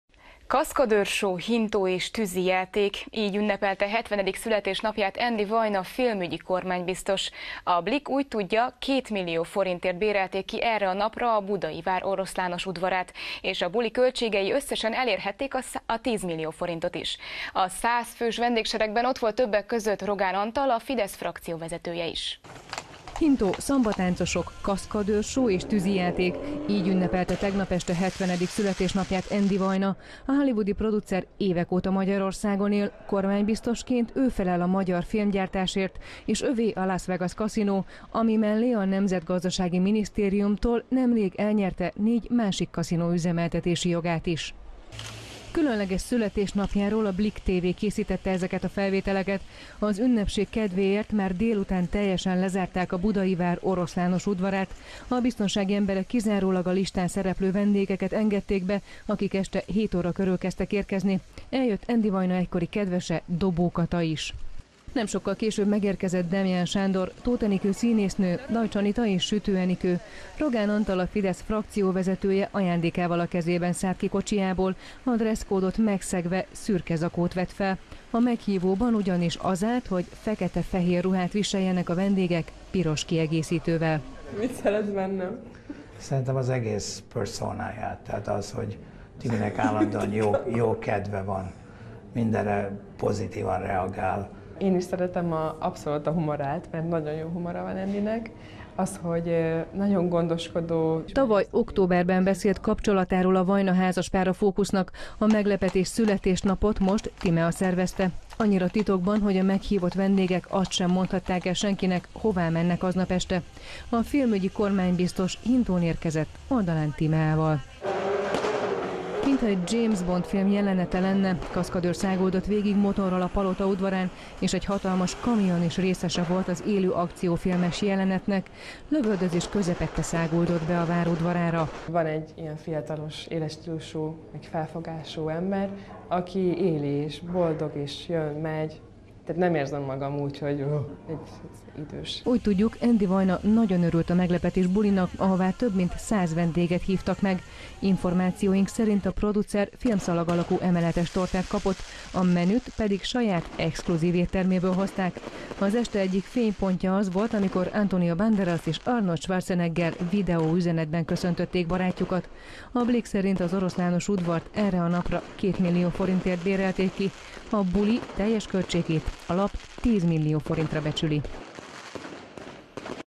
Andy Vajna a Várban ünnepelt 2014-08-02
Kaszkadőr show, hintó és tűzijáték – így ünnepelte 70. születésnapját Andy Vajna filmügyi kormánybiztos. A Blikk úgy tudja, 2 millió forintért bérelték ki erre a napra a Budai Vár Oroszlános udvarát.